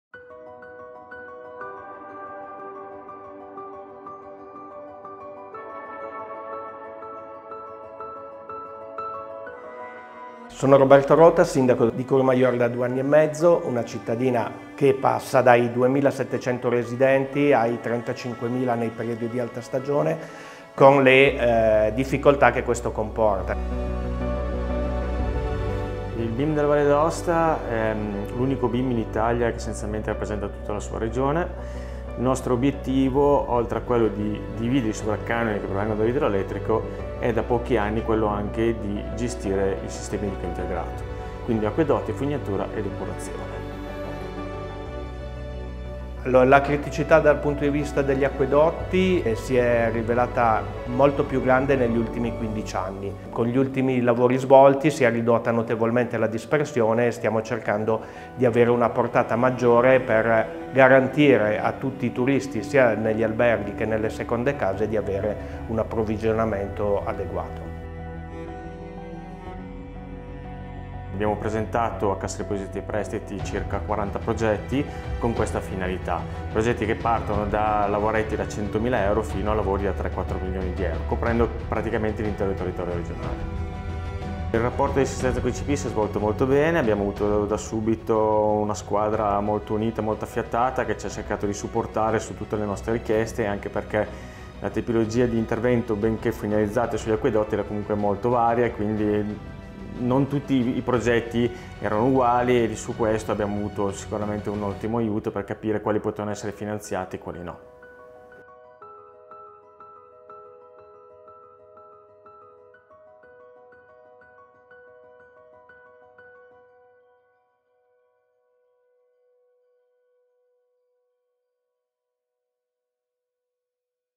storiedallitalia: Valle d’Aosta, 30 milioni per investire nel sistema idrico con il Prestito green
Con un finanziamento di 30 milioni numerosi Comuni della Valle d’Aosta potranno realizzare interventi per migliorare il sistema idrico integrato.

Il prestito è stato concesso da CDP a favore del Consorzio BIM (Bacino Imbrifero Montano), l’ente che gestisce nei 74 Comuni della Regione tutte le fasi del trattamento dell’acqua: dai servizi di acquedotto a quelli di fognatura e depurazione.

Saranno finanziati oltre 40 progetti di diversa dimensione su tutto il territorio della Valle d’Aosta per ridurre la dispersione e garantire un approvvigionamento adeguato per abitanti e turisti.  Iniziative in linea con l’obiettivo di favorire il risparmio idrico e prevenire gli effetti delle carenze di acqua sempre più frequenti.

Due terzi delle risorse, pari a 20 milioni, saranno erogati attraverso il “Prestito green”. Il prodotto è stato lanciato in collaborazione con la BEI per favorire gli investimenti sostenibili degli enti pubblici a condizioni finanziarie agevolate attraverso un plafond di 200 milioni. Gli interventi finanziabili rientrano in quattro ambiti prioritari: economia verde e risorse idriche, efficientamento energetico, trasporto pubblico e trattamento dei rifiuti.

L’operazione in favore del Consorzio BIM è solo l’ultima di un lungo impegno di CDP in favore del settore idrico: dal 2016 sono state infatti realizzate 360 operazioni per oltre 220 milioni con l’obiettivo di investire in acquedotti, dighe, opere di bonifica e reti fognarie.

L’iniziativa segue le priorità di intervento individuate nel Piano Strategico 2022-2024 dalle Linee Guida Settoriali sulla Salvaguardia del Territorio e della Risorsa Idrica per sostenere l’ammodernamento e il potenziamento delle infrastrutture.